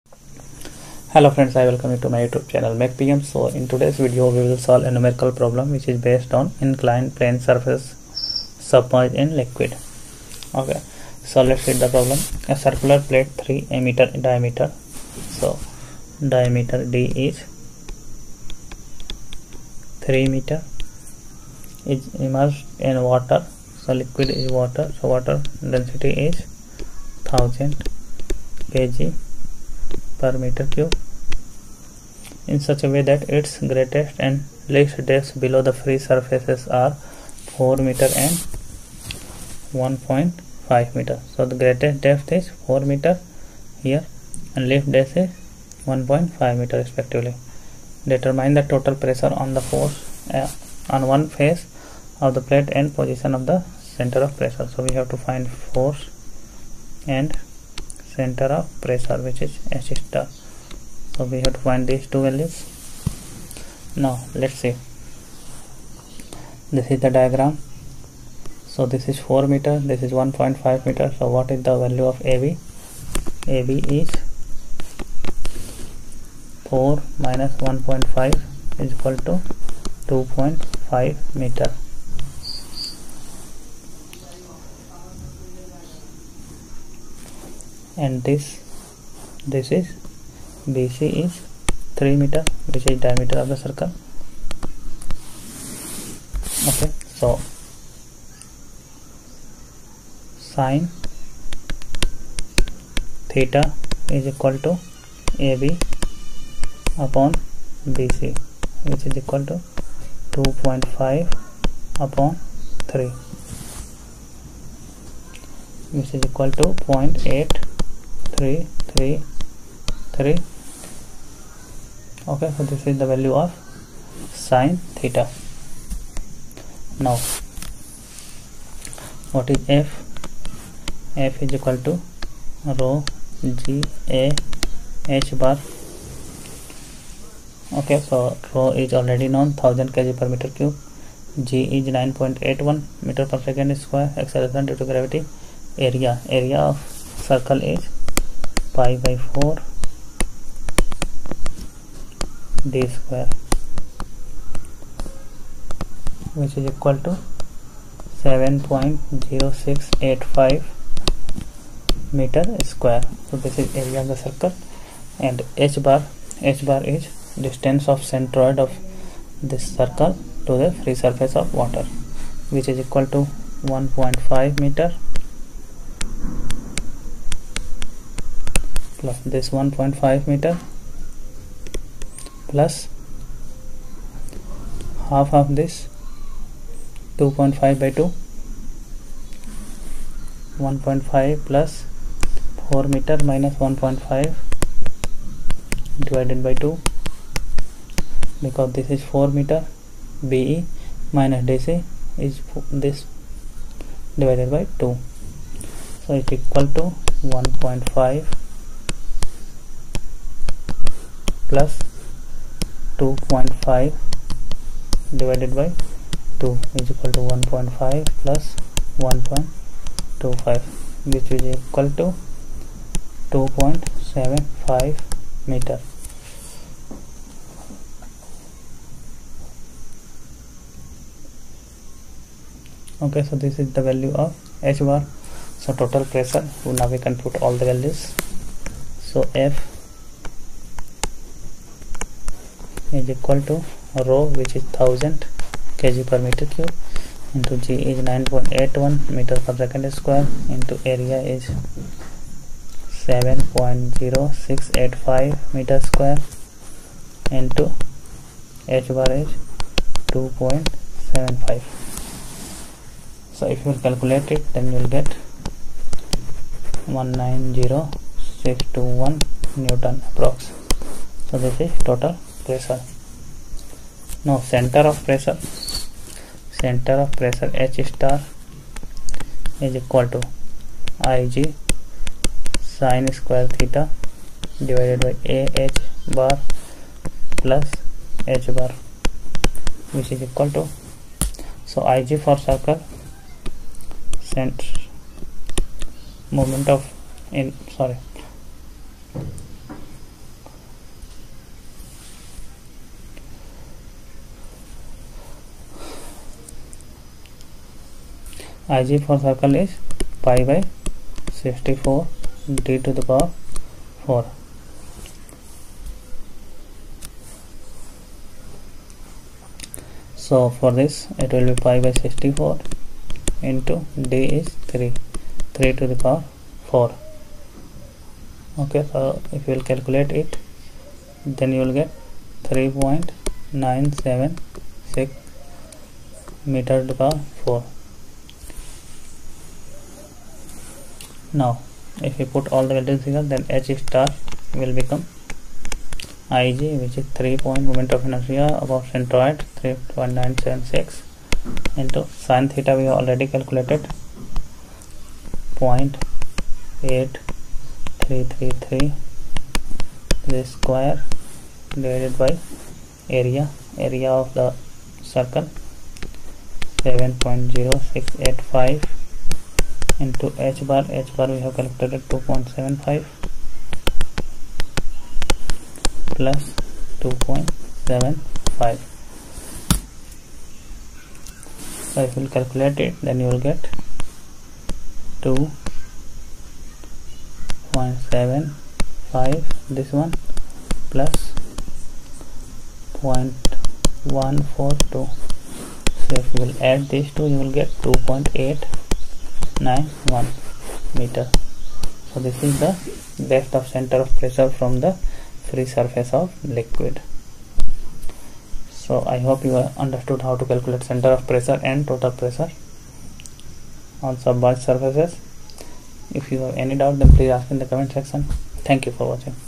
Hydrostatic forces on submerged Surfaces: 12: inclined circular plate
In this video we will try to find total pressure or force and center of pressure on inclined circular plate